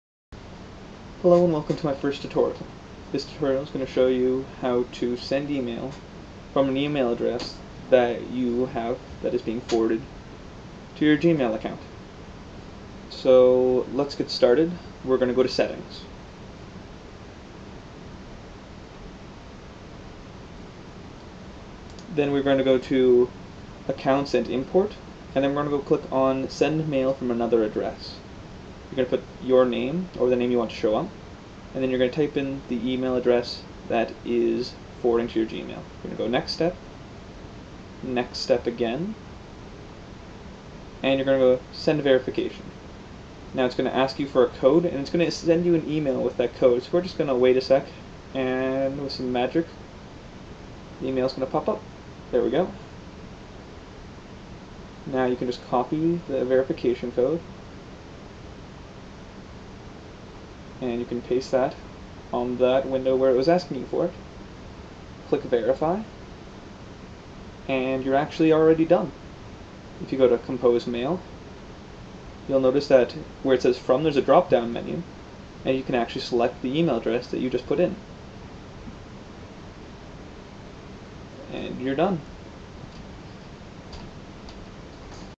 Gmail Forwarding Emails Tutorial.
KCcoding Tutorials

Lets say you have an email account but you love Gmail so you forward it to Gmail. Now what if you want to send an email from that other email account but don't want to leave Gmail? well here is how you do it.